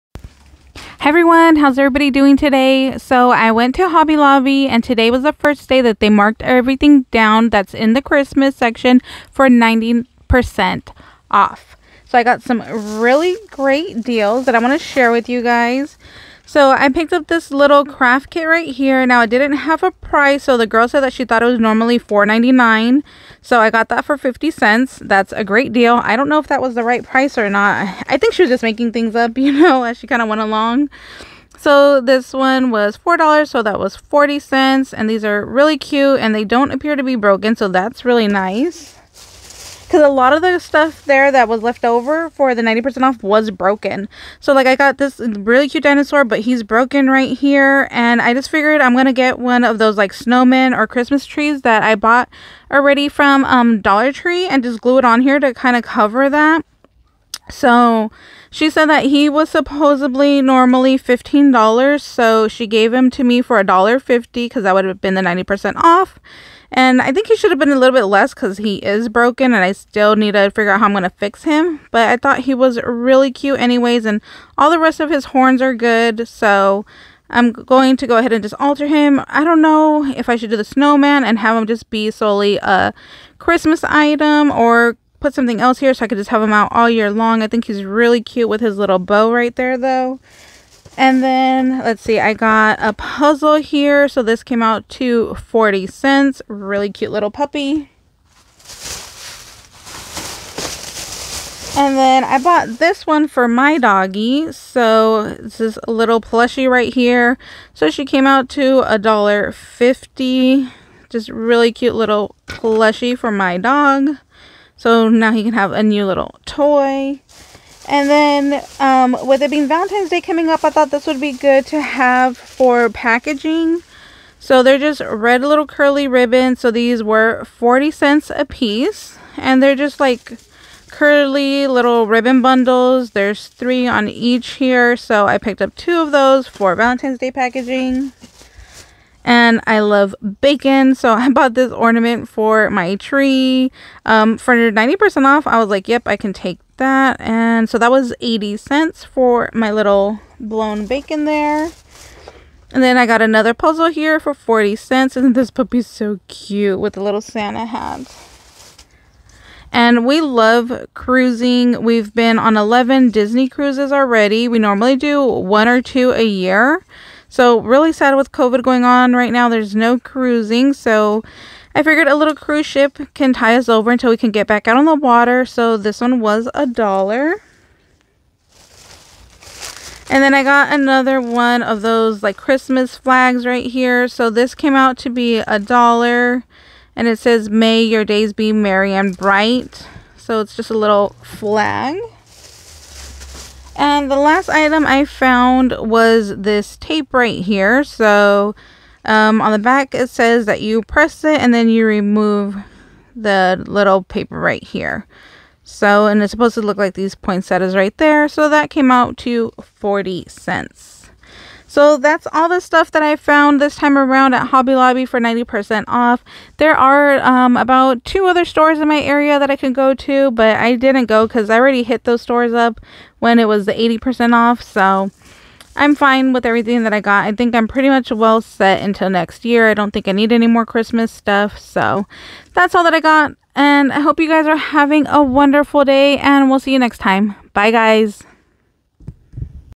Hobby Lobby 90% off Christmas Haul
Hi everyone today at Hobby Lobby the Christmas stuff went on clearance for 90% off I wanted to share what I was able able find